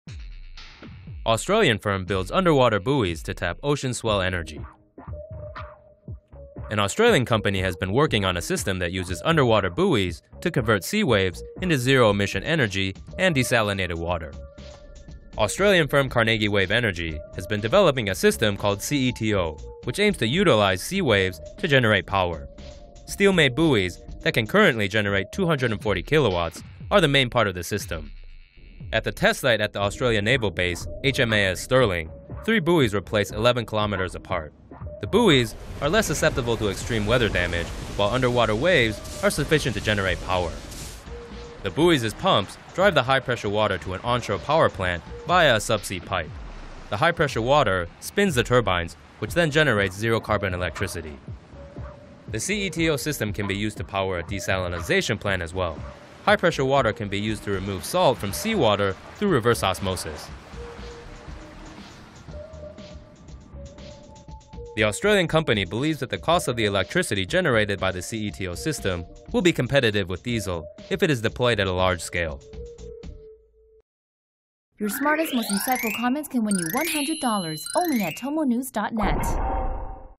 Renewable energy: CETO system converts sea waves into zero-emission electricity
An Australian company has been working on a system that uses underwater buoys to convert sea waves into zero-emission energy and desalinated water.

Australian firm Carnegie Wave Energy has been developing a system called CETO, which aims to utilise sea waves to generate power. Steel-made buoys that can currently generate 240 kilowatts are the main part of the system.

At the Australian naval base HMAS Stirling, three buoys are placed 11 kilometres apart. The buoys are less susceptible to extreme weather damage, while underwater waves are sufficient to generate power.

The buoys' pumps drive the high pressure water to an onshore power plant via a subsea pipe. The high pressure water spins the turbines, which then generate zero-carbon electricity.

The CETO system can be used to power a desalination plant as well. High pressure water can be utilised to remove salt from seawater through a permeable membrane in a process called reverse osmosis.

The Australian company believes that the cost of electricity generated by the CETO system will be competitive with diesel if it is deployed at a large scale.